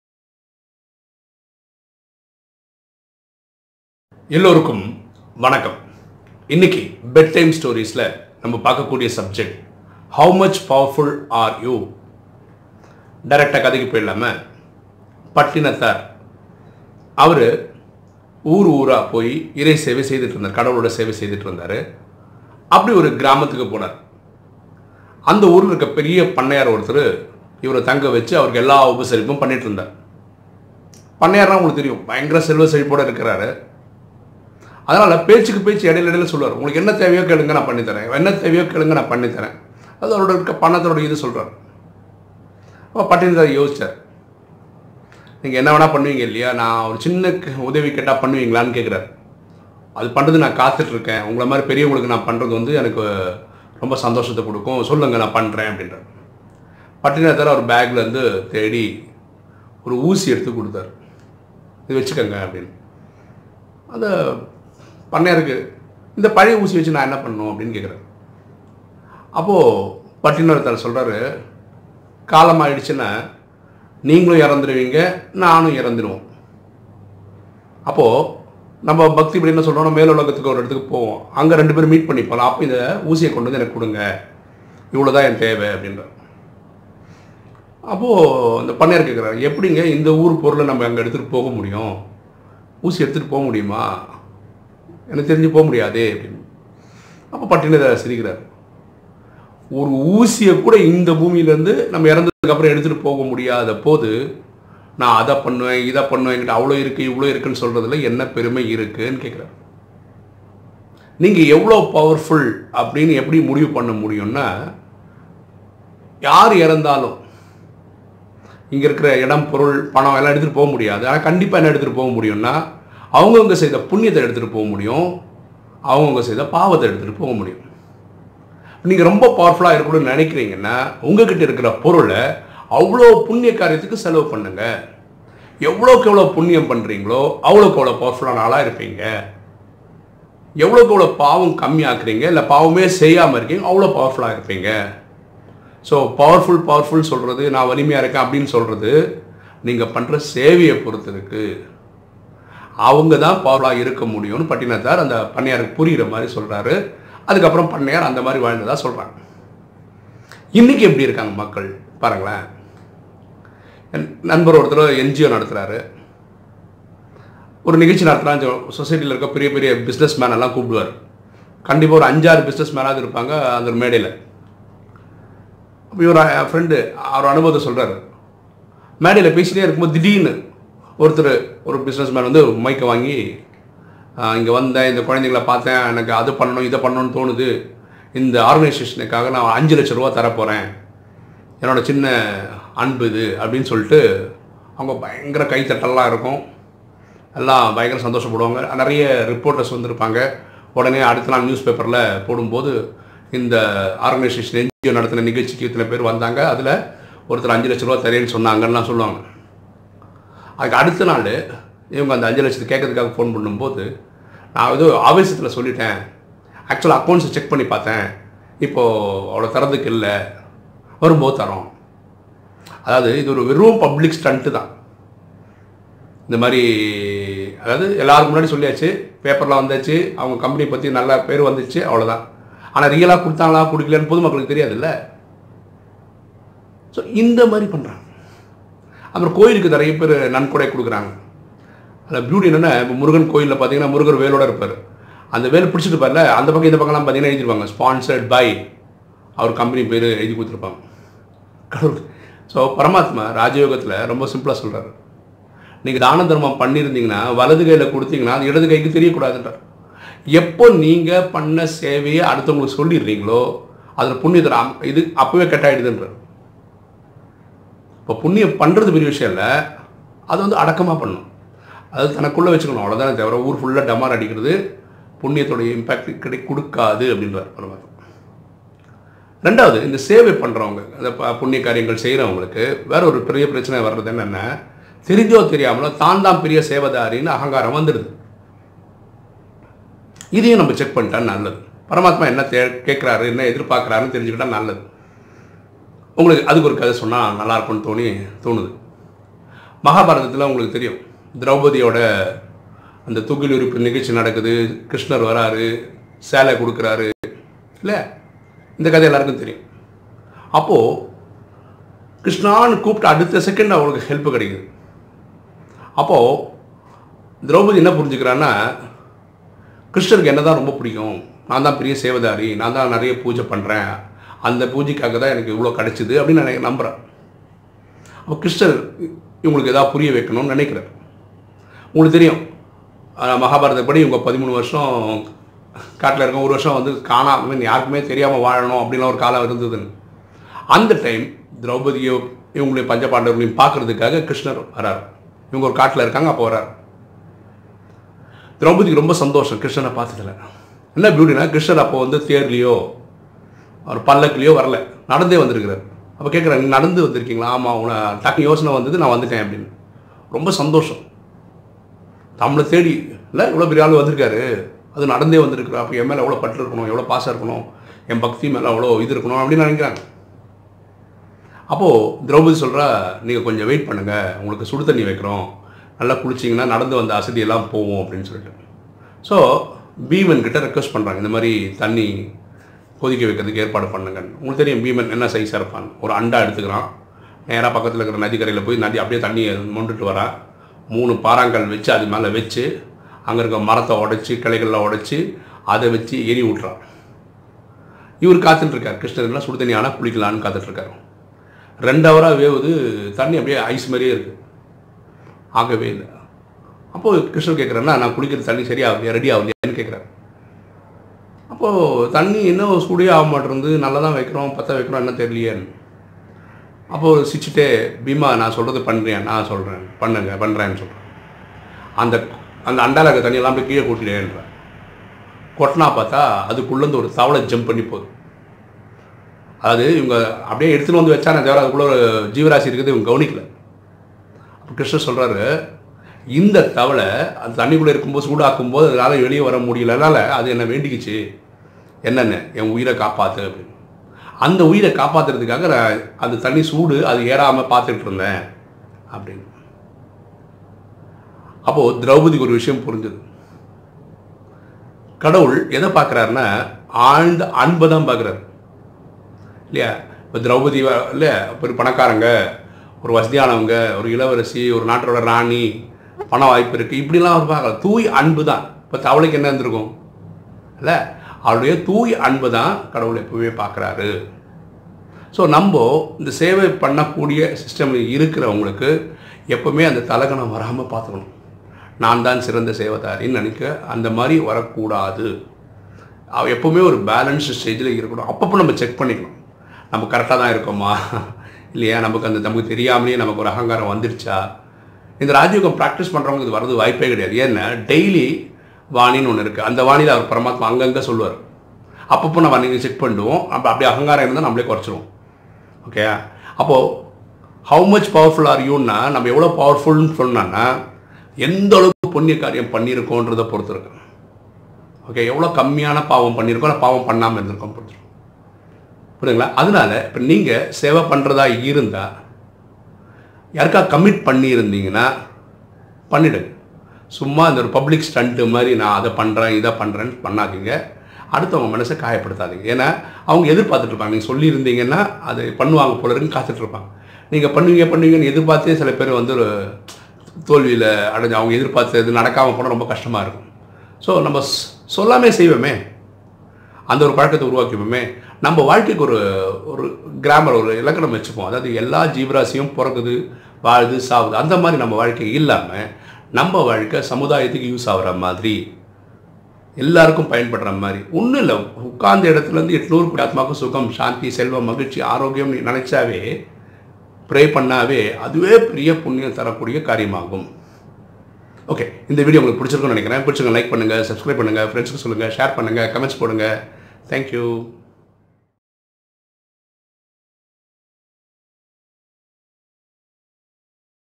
How much Powerful are YOU? | Bed Time Stories | Tamil | Series #63
Watch a story about Pattinathar to know how much powerful you are?

for more videos in our YouTube Channel.